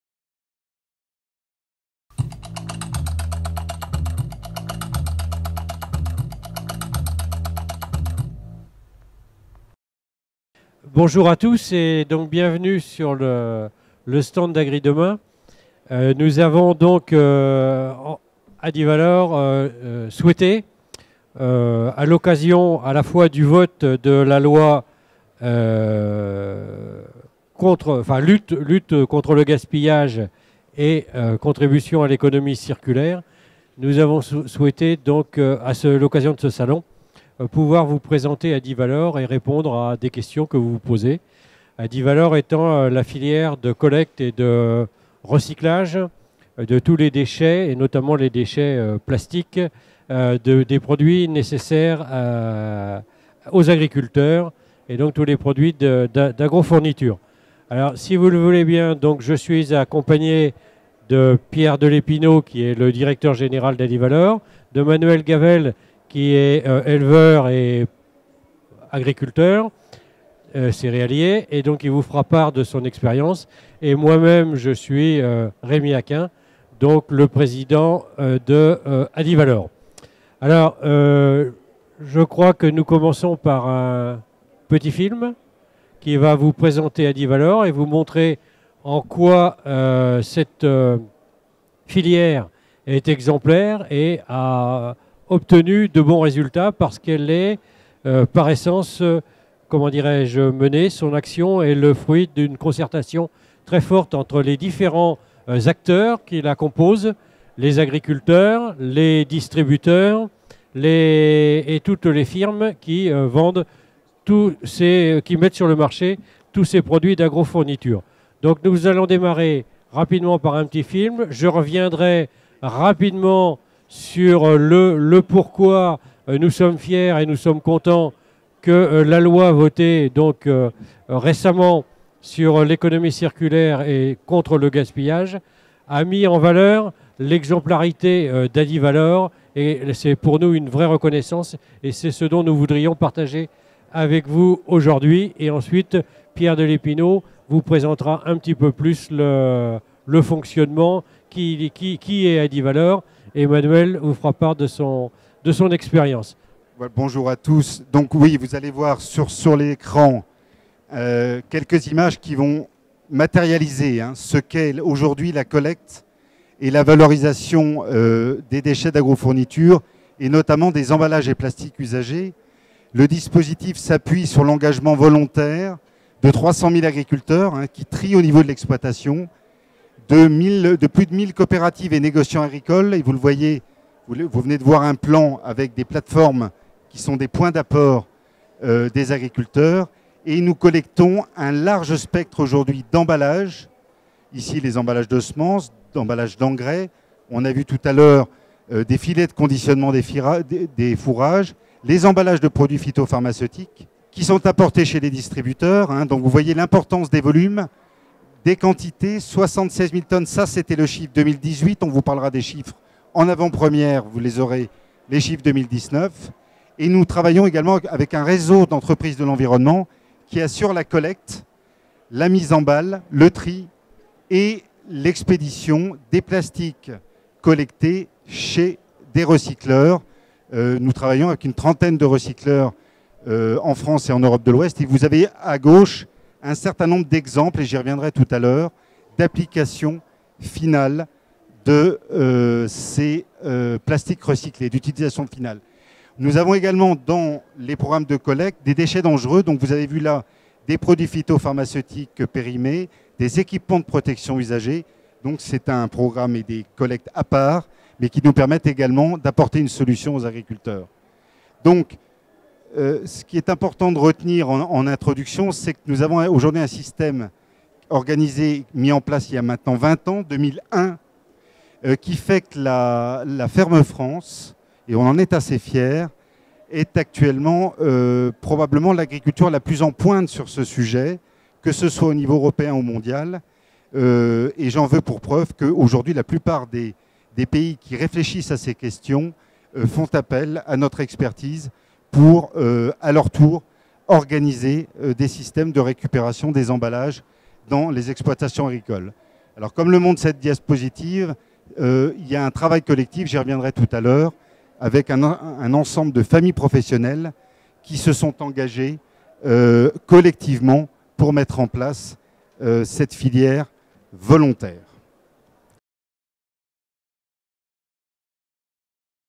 L'agriculture française championne du monde du recyclage
Rémi Hacquin, président d'Adivalor, présente rapidement la filière de recyclage des plastiques usagés agricoles, mettant en avant l'exemplarité de la France en la matière, montrant ainsi que ce secteur est un acteur essentiel de l'économie circulaire.